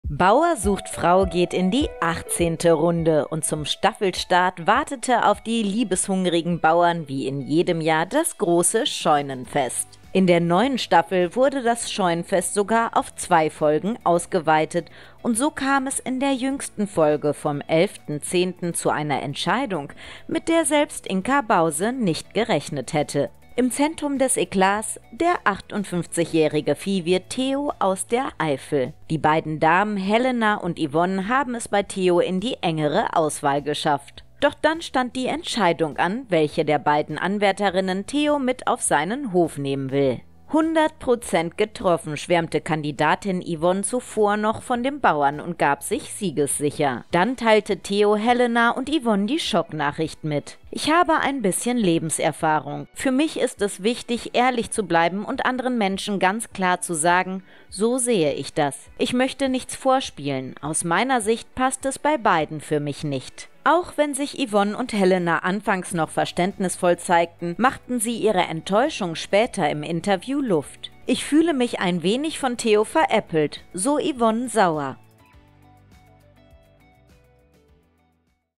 Eklat bei "Bauer sucht Frau": Sogar Inka Bause entsetzt!
Die neue Staffel "Bauer sucht Frau" hat gerade erst begonnen und schon kam es zu einer Entscheidung, mit der wirklich niemand gerechnet hätte!